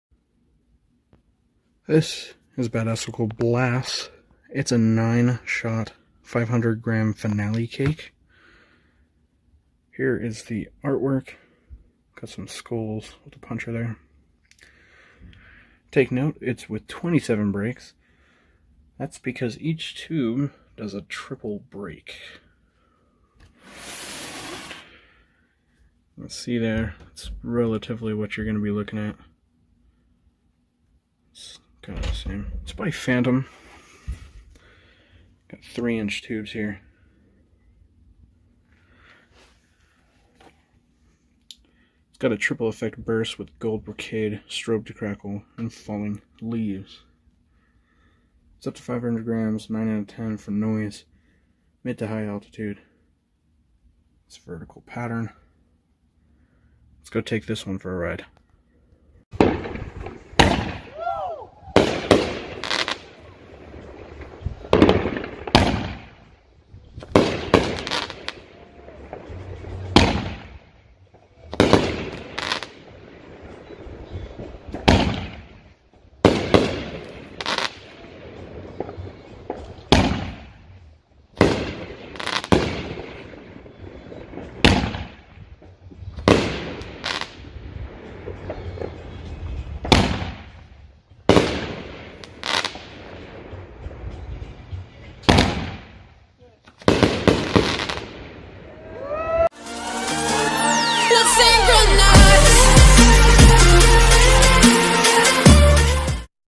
Badassical Blast By Phantom Fireworks
500 Gram NOAB Cake with 9 Shots.
By Phantom Fireworks.

0:00 Explanation
0:59 Demo